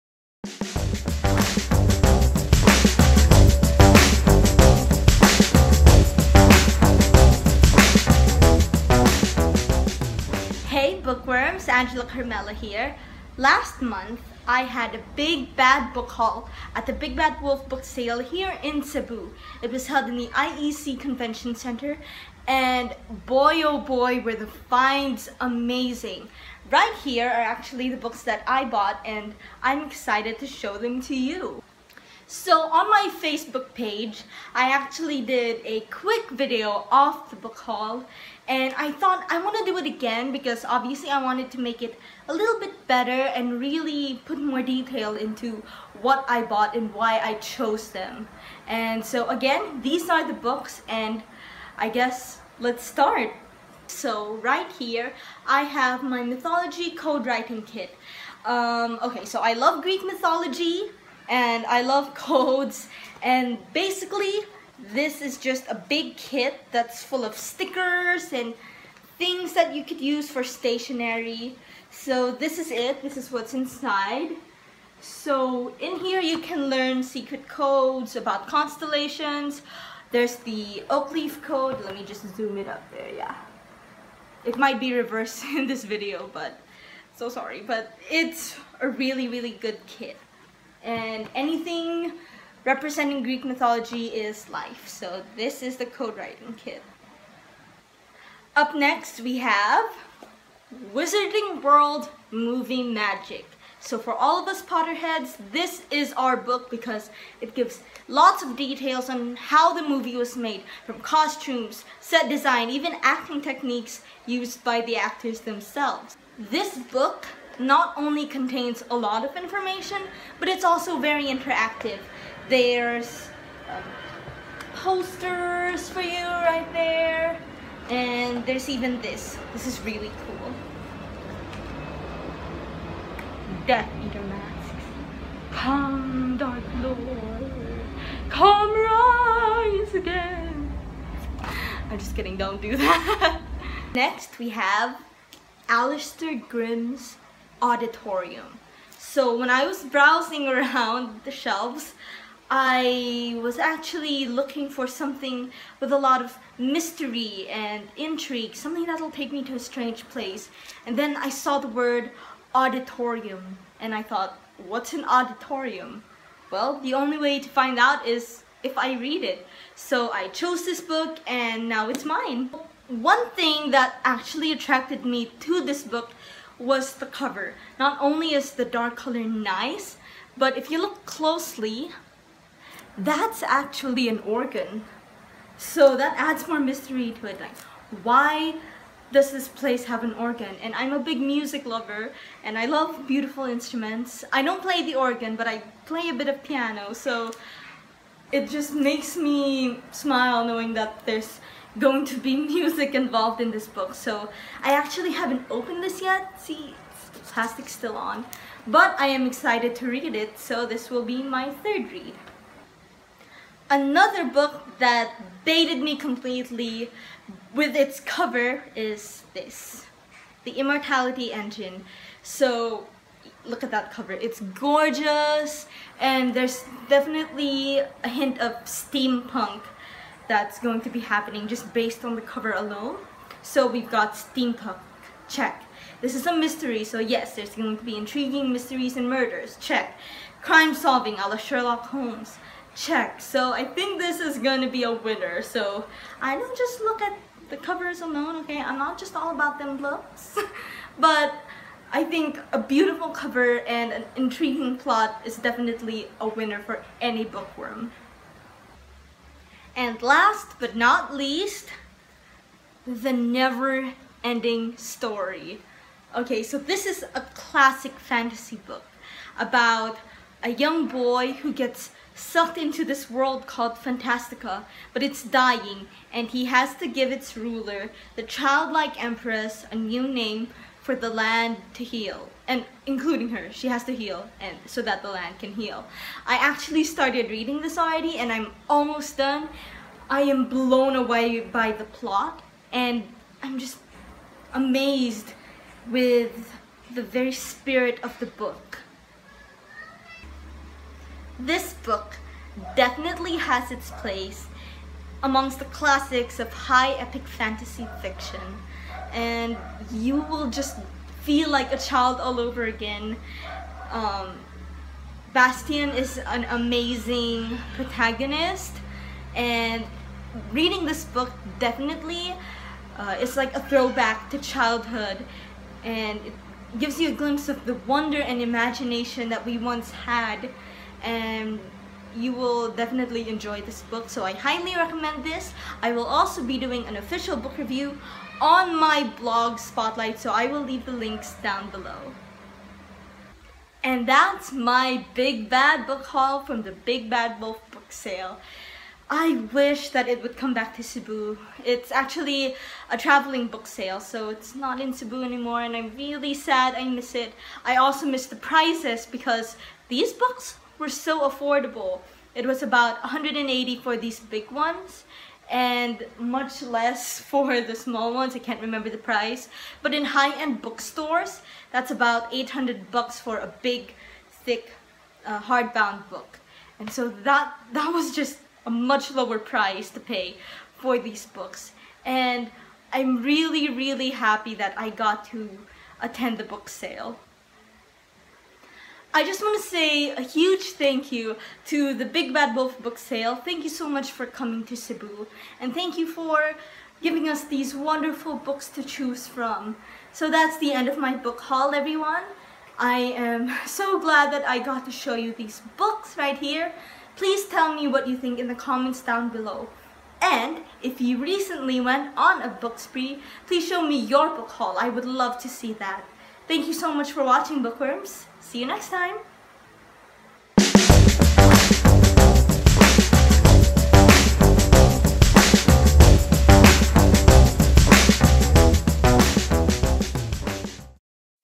Big Bad Book Haul @ Big Bad Wolf Book Sale
The Big Bad Wolf Book Sale came to Cebu, and I ended up with some exciting new additions to my collection. Here's my Big Bad Book Haul!

Here's the link to the original haul video I made the day I bought the books: ‪